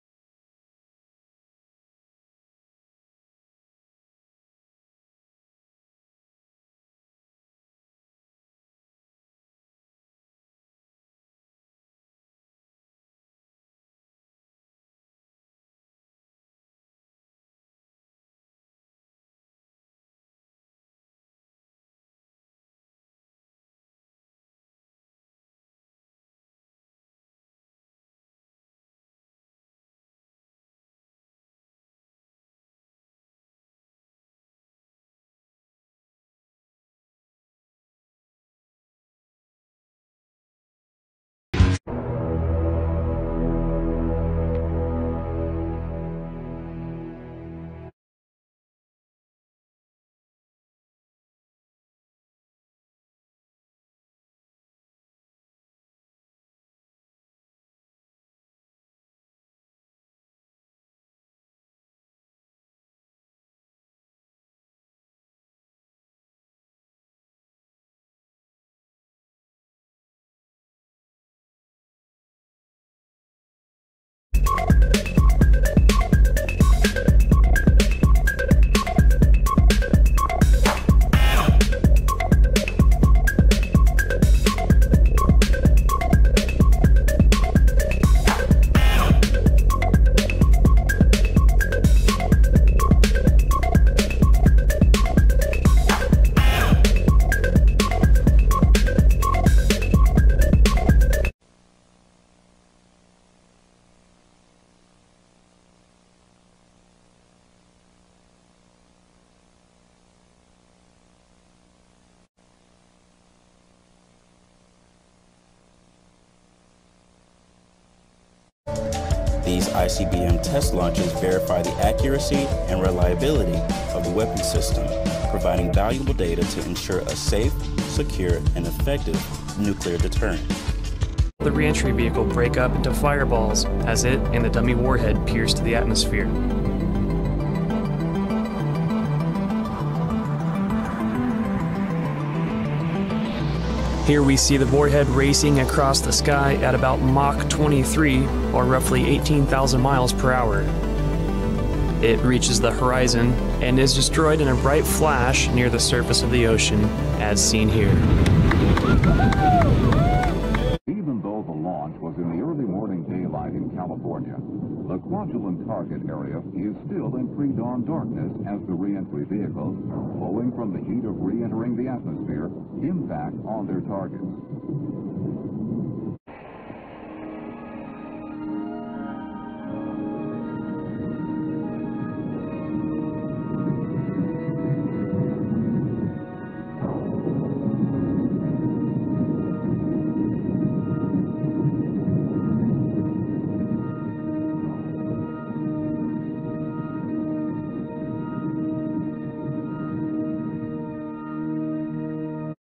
Intercontinental Ballistic Missile (ICBM) - MIRV Reentry Compilation | Part II
Second compilation video of Intercontinental Ballistic Missiles' (ICBM) reentry phase. Peacekeeper and Minuteman missiles are included in the video.
Videos are from "Association of Air Force Missileers" and "U.S. Army Garrison Kwajalein Atoll" channels.